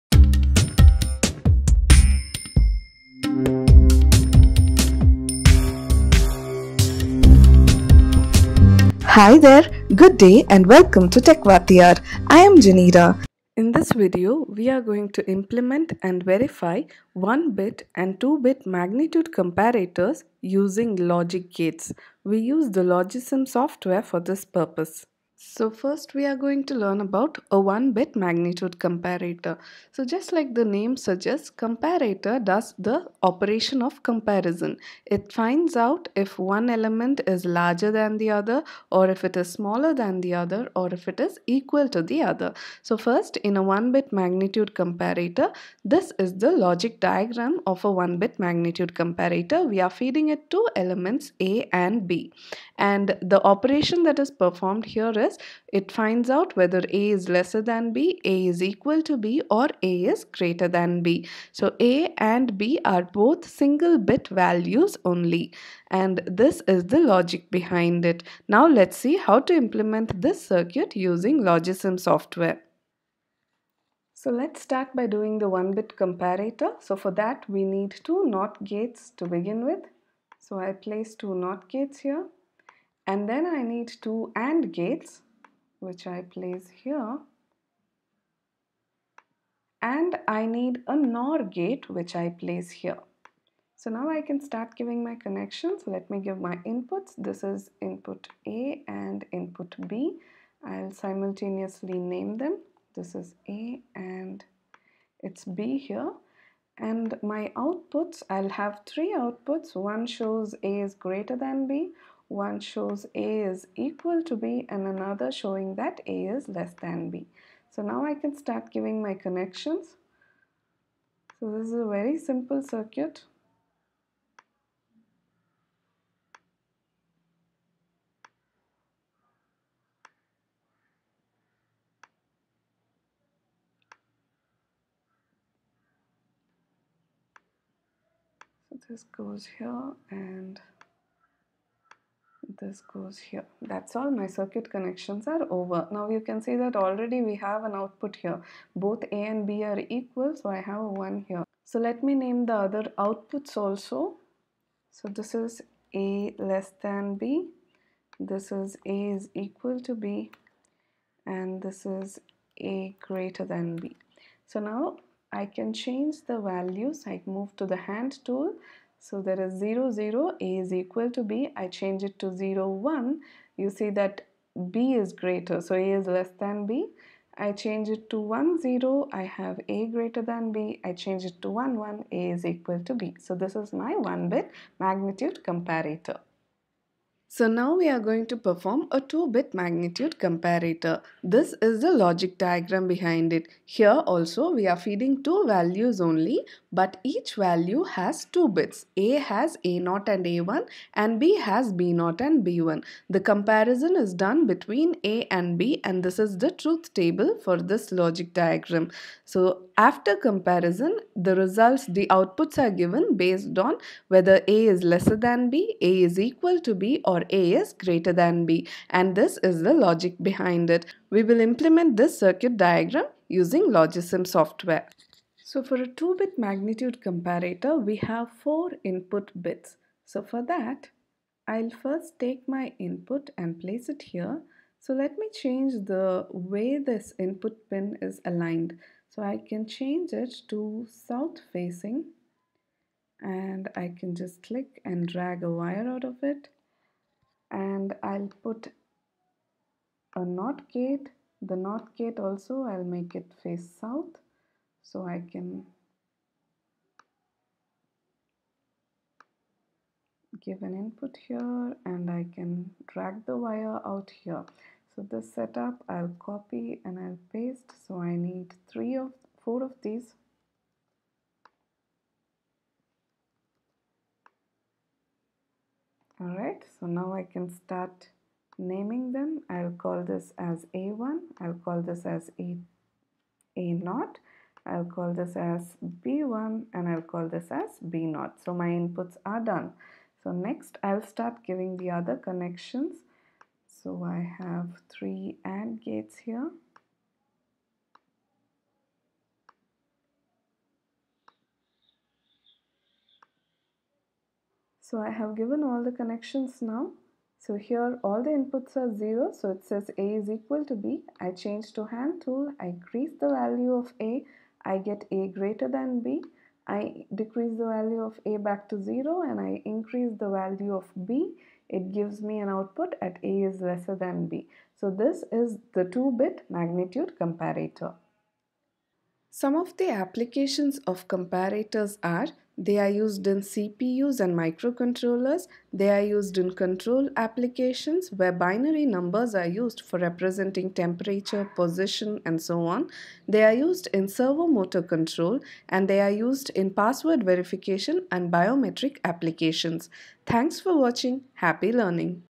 Virtual Lab - Comparators using Logisim
Implementation and Verification of the truth table of one-bit and two-bit magnitude comparator using logic gates